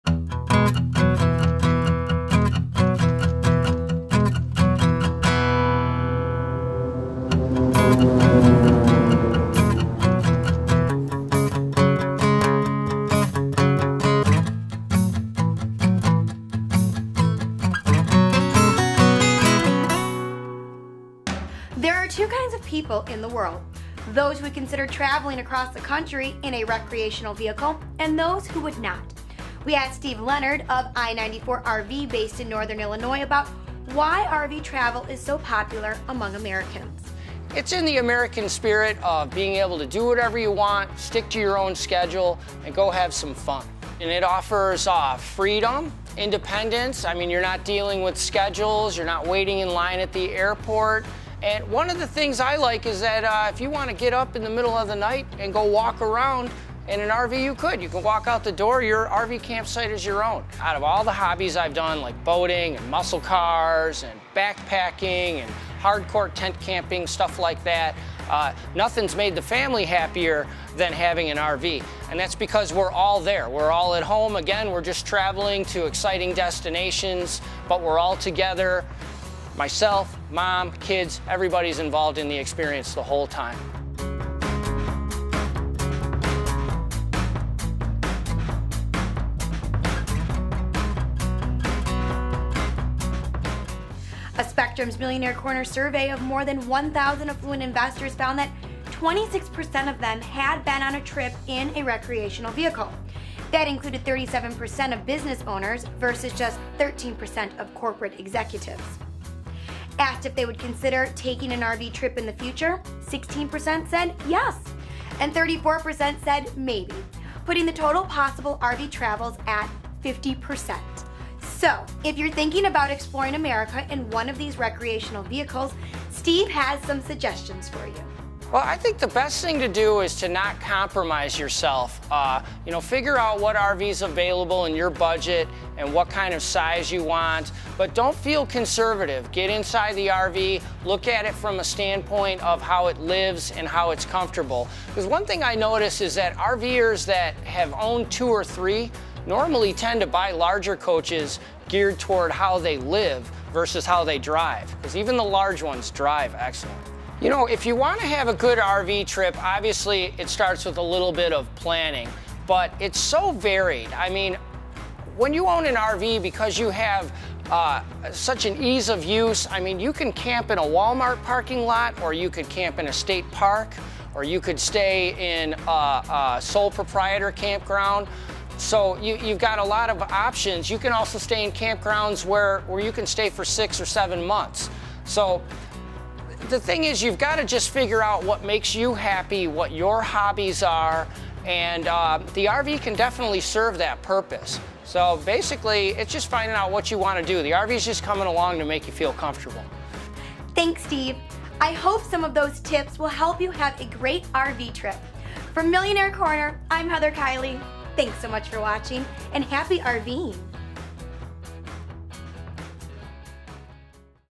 The Popularity of RV Travel - Millionaire Corner
"More than 20 million Americans travel in RVs every year because of the unmatched freedom and flexibility they provide when traveling,'' said RVIA President Richard Coon in an industry press release from June. "RVers can adjust their plans to take a long road trip or a quick weekend getaway. An RV is perfect for what many people want most: more quality time outdoors with loved ones."

A Spectrem's Millionaire Corner survey of more than 1,000 affluent investors found that 26 percent of them had been on a trip with a recreational vehicle. That included 37 percent of business owners (versus just 13 percent of Corporate Executives).

Asked if they would consider taking an RV trip in the future, 16 percent said "yes'' and 34 percent said "maybe'', putting the total possible RV travelers at 50 percent. Income played a role, as 57 percent of investors with a household income of under $100,000 said yes or maybe, while only 37 percent of those with a household income over $200,000 said they would consider it or do it.